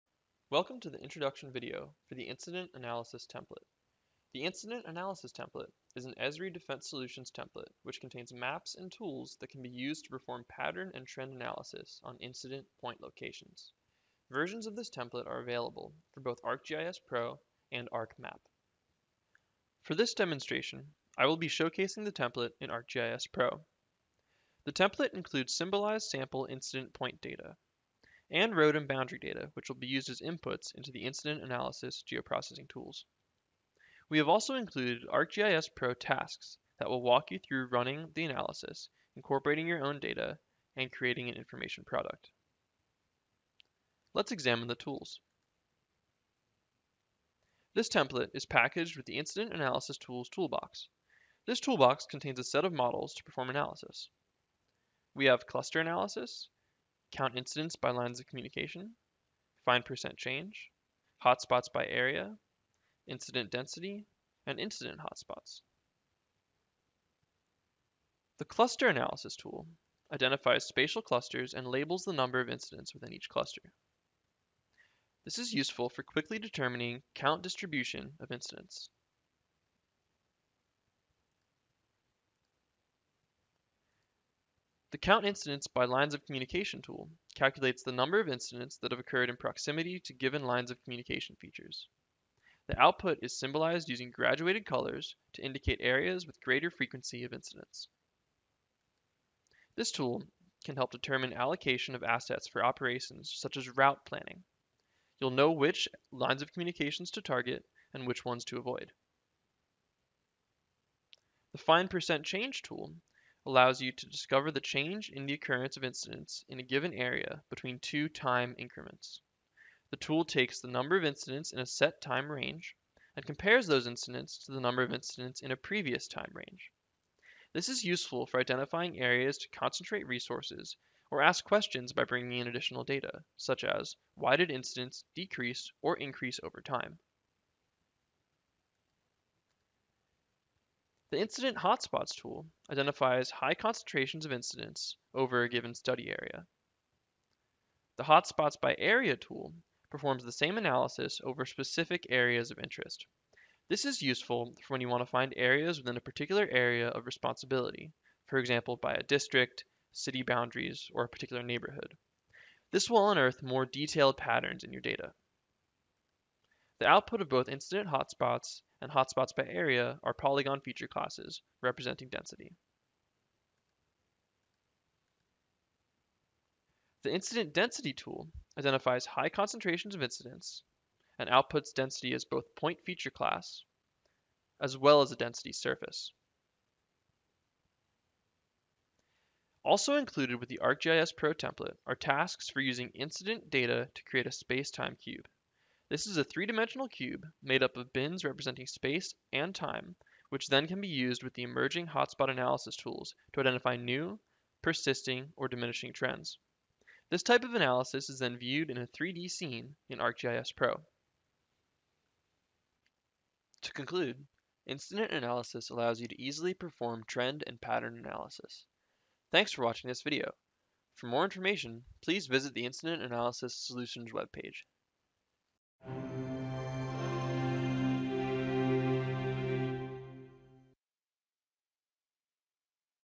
Incident Analysis for ArcGIS Pro: Introduction
The Incident Analysis template is an Esri Defense Solutions template which contains maps and tools that can be used to perform pattern and trend analysis on incident point locations. Versions of this template are available for both ArcGIS Pro and ArcMap.

The Incident Analysis template is the starting point to discover spatial patterns and trends in significant activity in your area of interest. The tools to help you do this are included in this template:

• Cluster Analysis
• Count Incidents by Lines of Communication
• Find Percent Change
• Hot Spots by Area
• Incident Density
• Incident Hot Spots
• Incident Table to Point